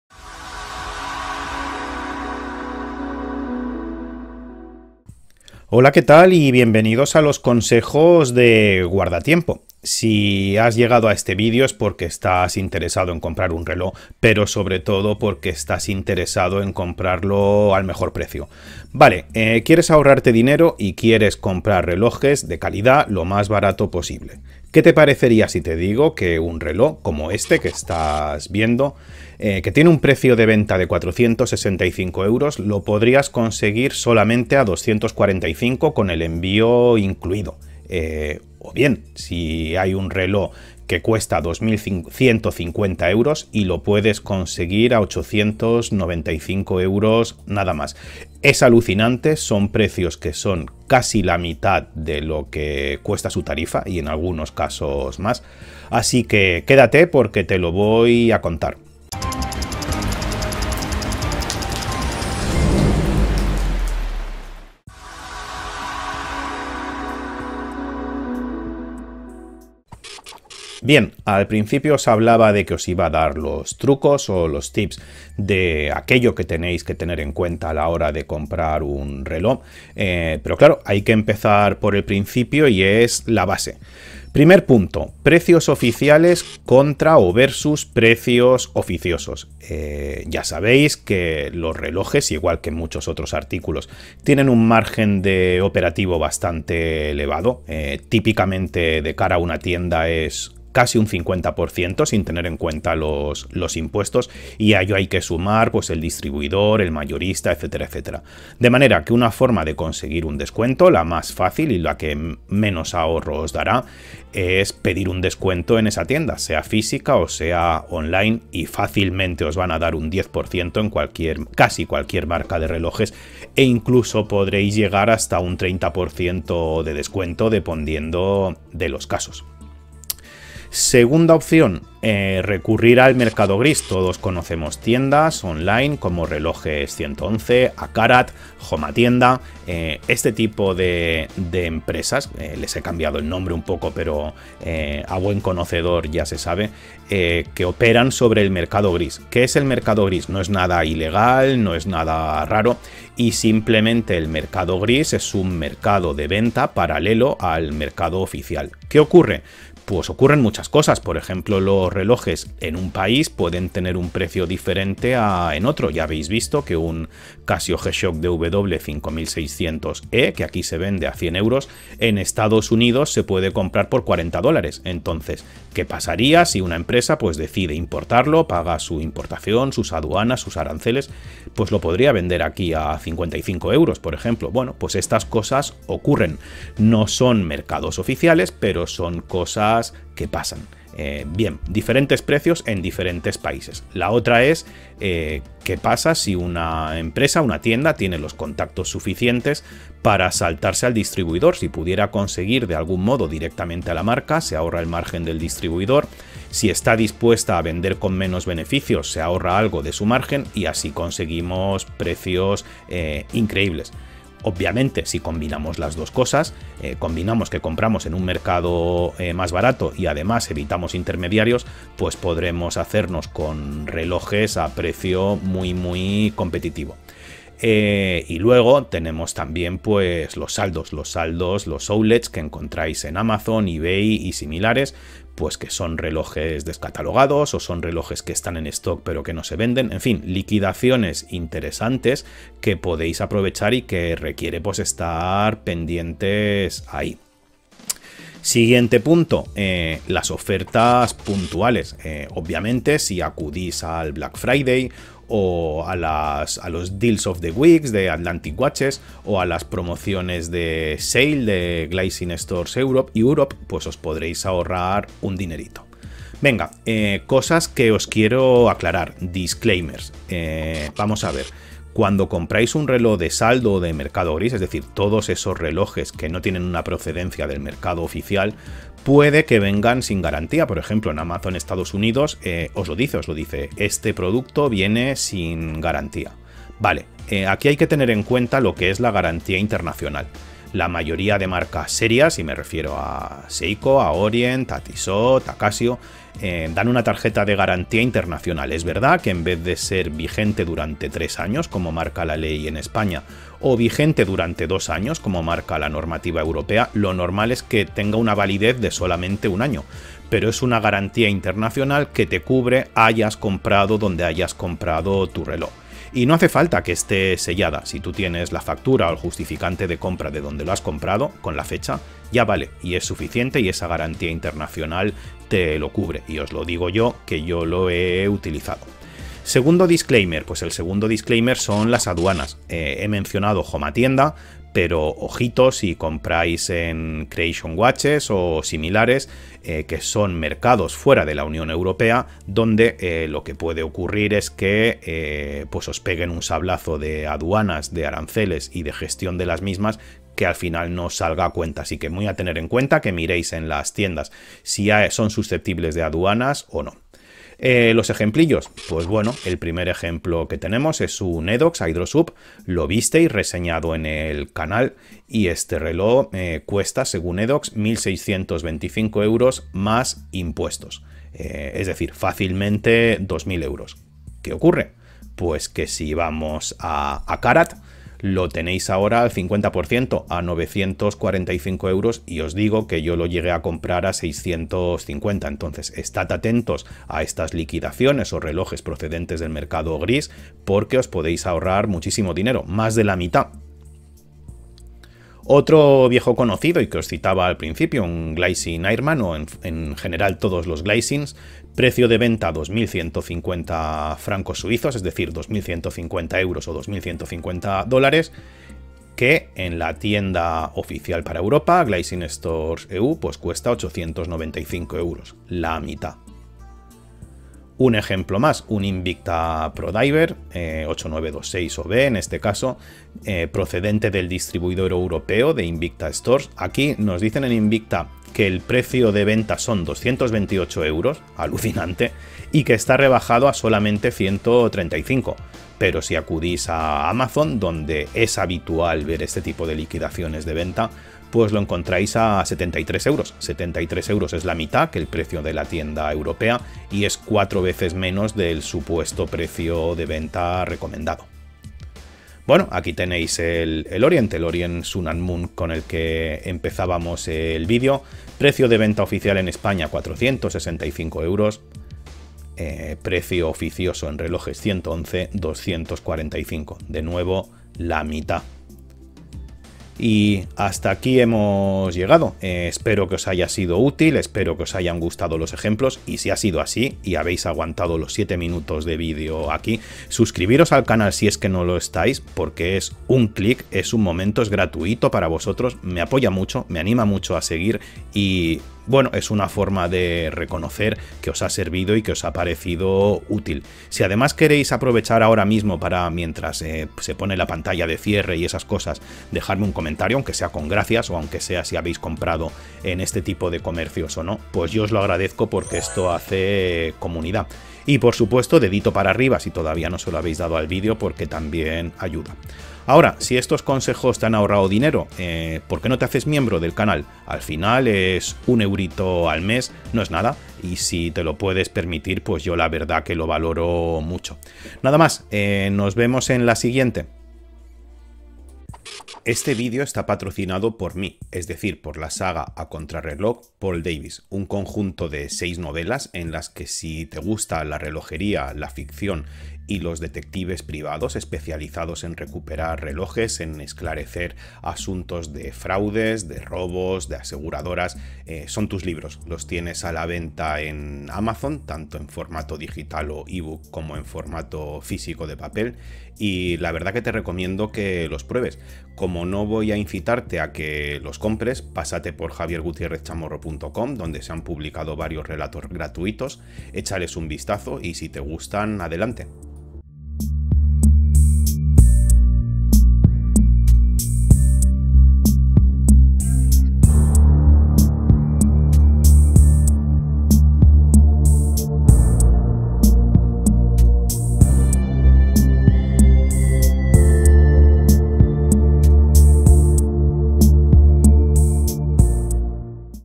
Cómo COMPRAR un RELOJ al MEJOR PRECIO
¿Quieres saber cómo ahorrarte dinero y comprar relojes al mejor precio? ¿Qué te parecería que un reloj que cuesta 465€ lo puedas conseguir a solamente 245€? ¿Y si un reloj que se vende a 2150 euros lo pudieras conseguir a sólo 895?

En este vídeo te mostraré todos estos trucos o tips, y te explicaré aquello que debe tener en cuenta.

- Mercado gris (Relojes111, ACarat, JomaTienda, ...)
- Saldos (Amazon, eBay, ...)
- Ofertas puntuales: Black Friday, Atlantic, Glycine, ...
- Garantía internacional
- Aduanas